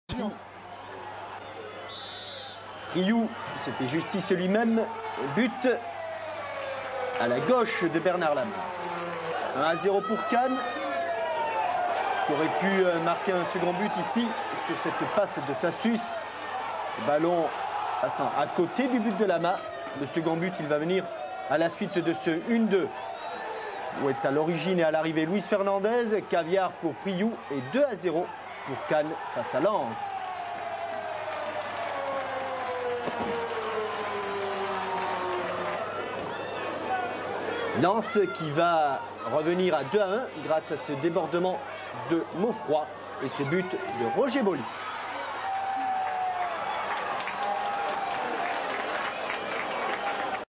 [Résumé] 04/04/1992 - Division 1 - Journée 34 - CANNES LENS 2 - 1
Cannes - Lens L'année de la remontée ...

Buts Lensois :
- Christophe Delmotte (80)

Buts Cannois :
- Franck Priou (30 pén., 67)